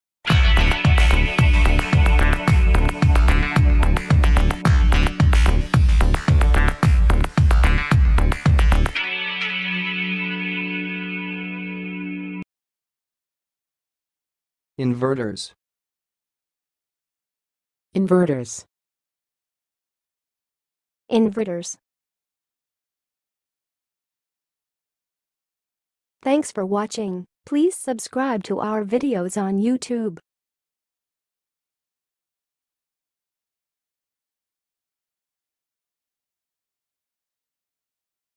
How to say "inverter's"! (High Quality Voices)
Watch in this video how to say and pronounce "inverter's"!